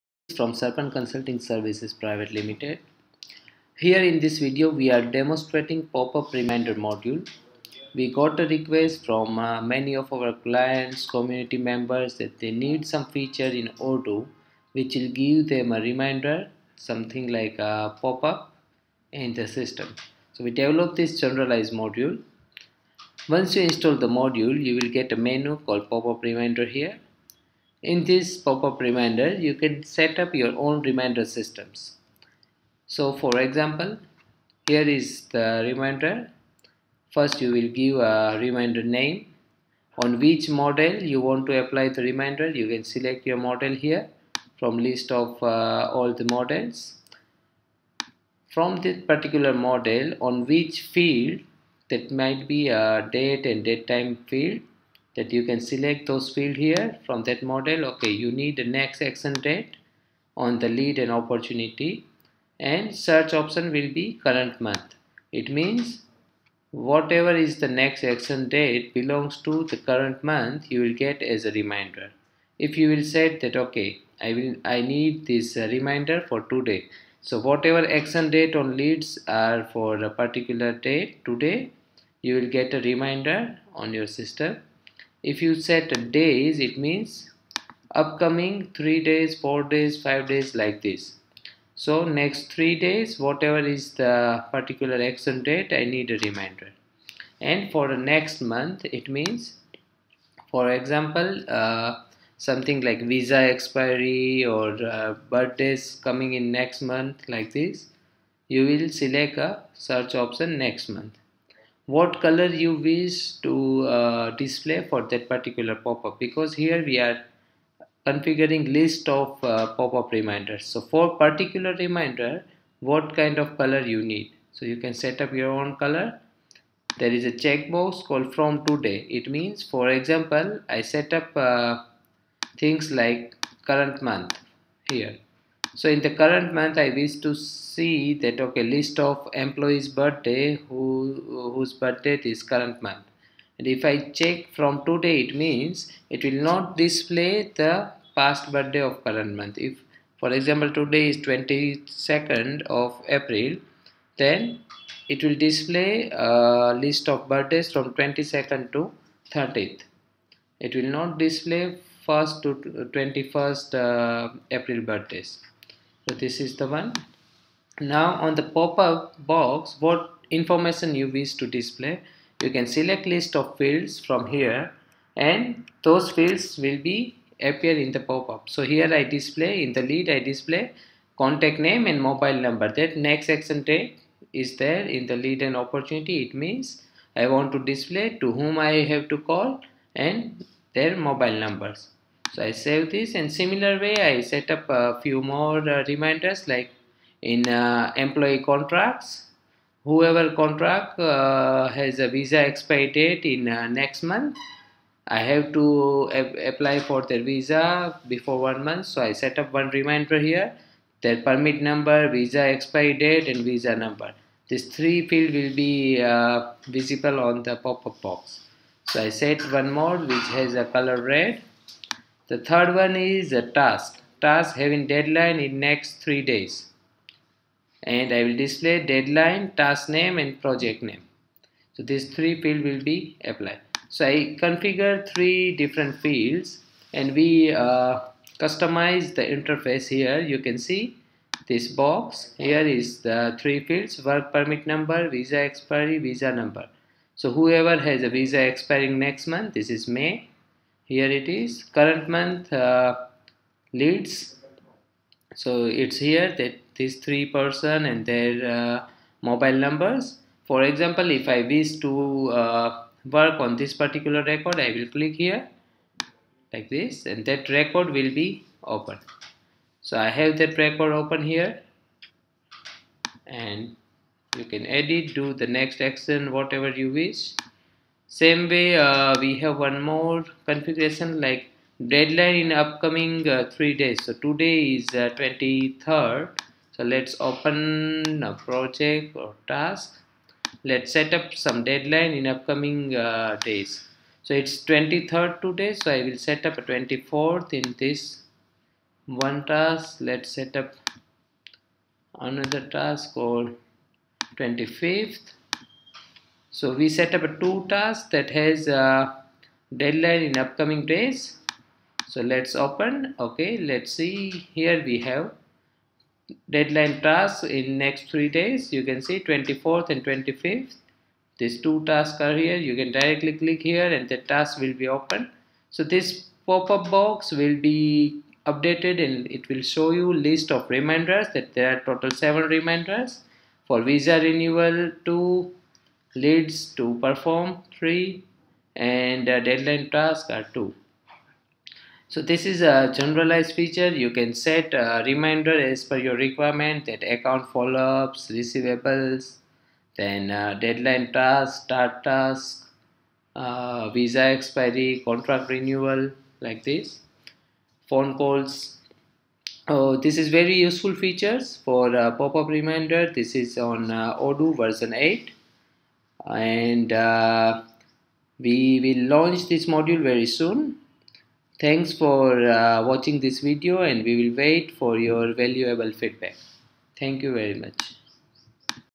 ODOO Popup Reminder Module
ODOO Module to setup reminders and configure fields for popup and get popup information on top status bar.